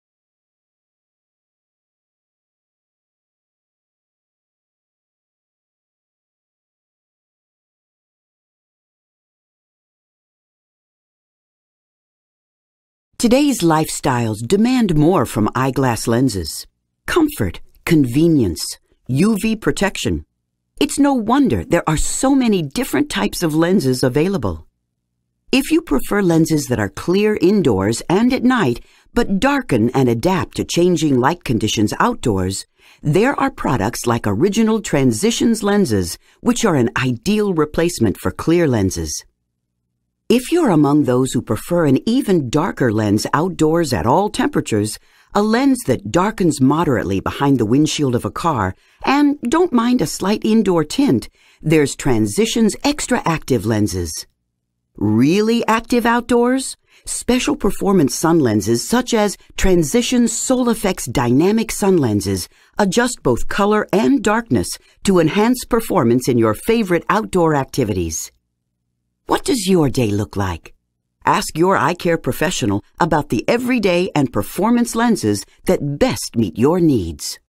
Transitions/Photochromic Lenses Explained
A short video which explains what a transitions/photochromic lens is and the benefits of having them.
for more information & Transitions Lenses, Reactolights Explained
Online Opticians UK